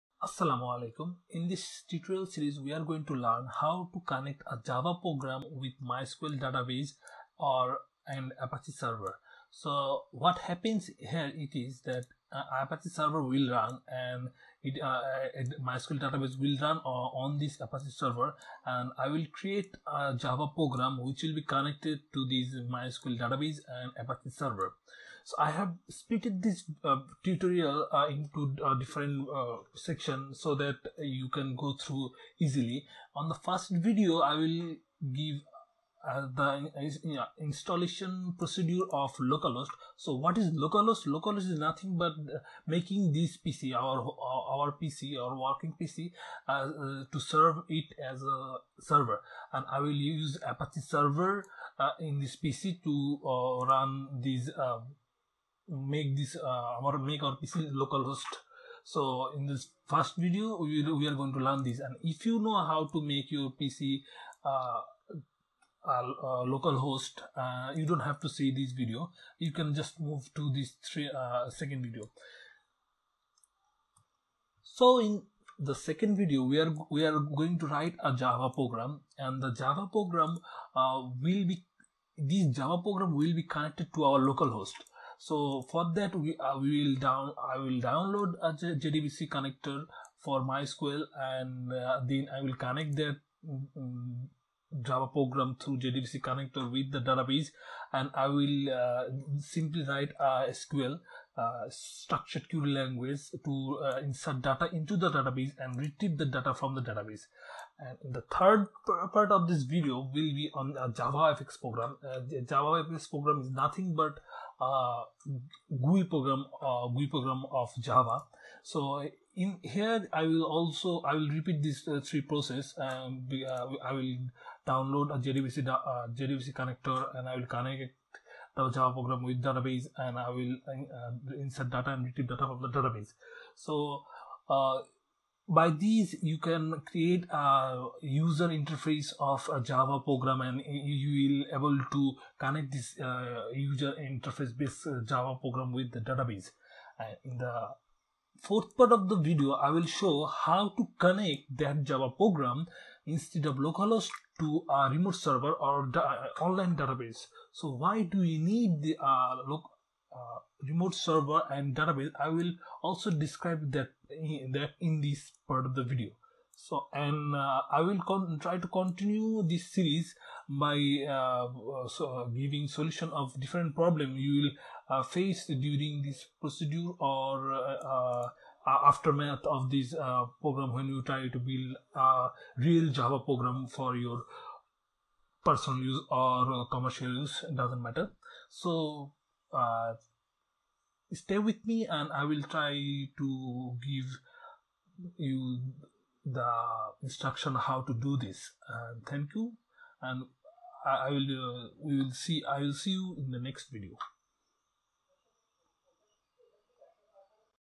Intro: How to connect java program with mysql Database/apache server
How to connect java program with MySQL Database/apache server
 Localhost installation
 Write Java Program
  Mysql JDBC Connector Download
  Connect the java program with database
  insert Data/retrieve data from Database
 Write JavaFx Program
  JDBC Connector Download
 Connect Java Program with Remote server/Database